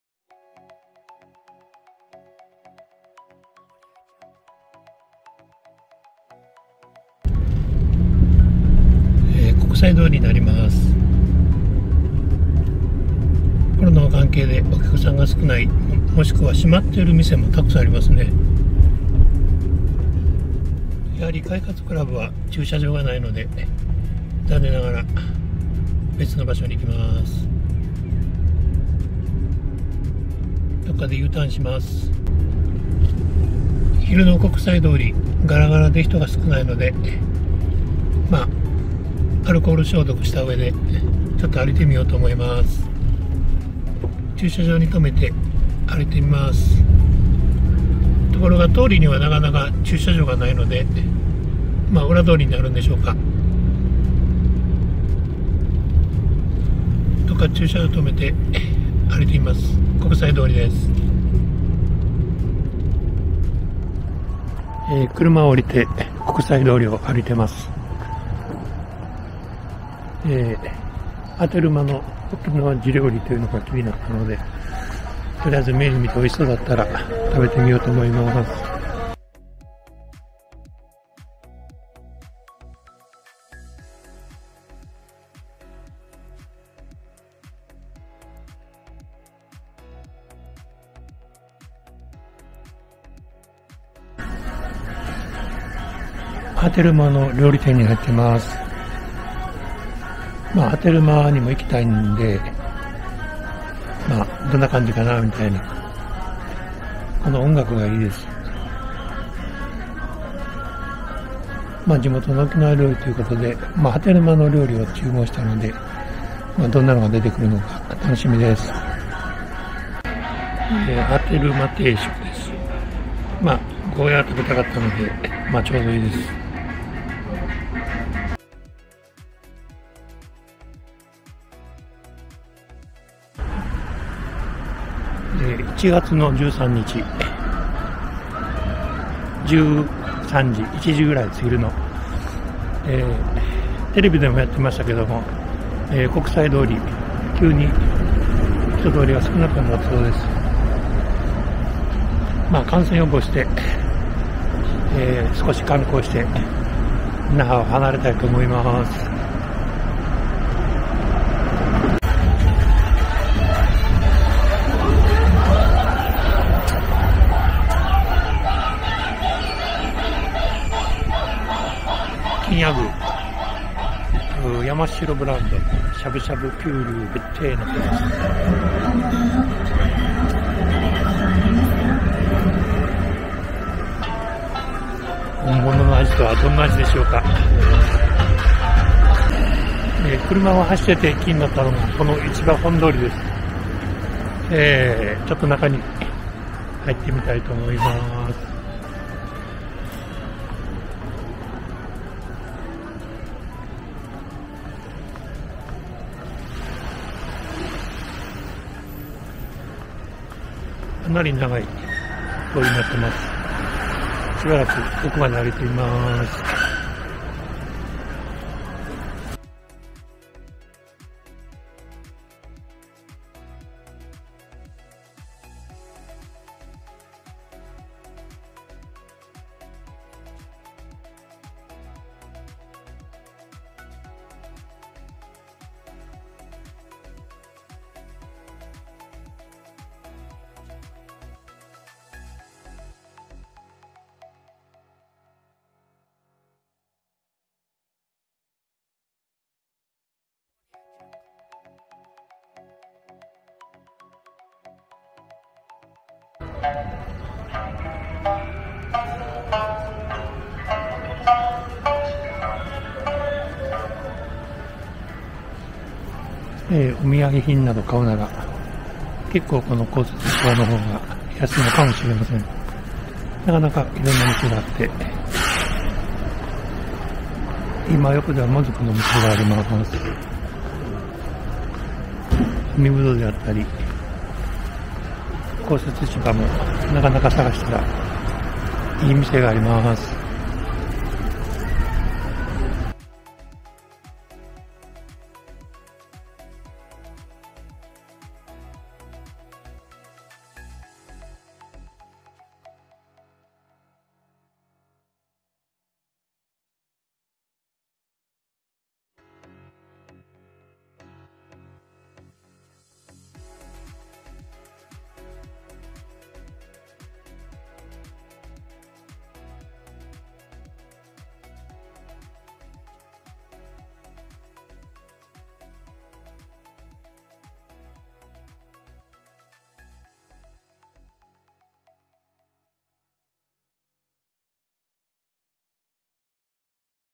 沖縄 国際通り+市場本通
コロナの関係で那覇を離れていましたが、国際通りの人通りが少ないと言うテレビの放送が有ったのでやって来ました。1月正月明けとコロナの関係で観光客は本当に疎らでした。市場本通りは楽しい店が一杯並んでいます。是非散策してみて下さい。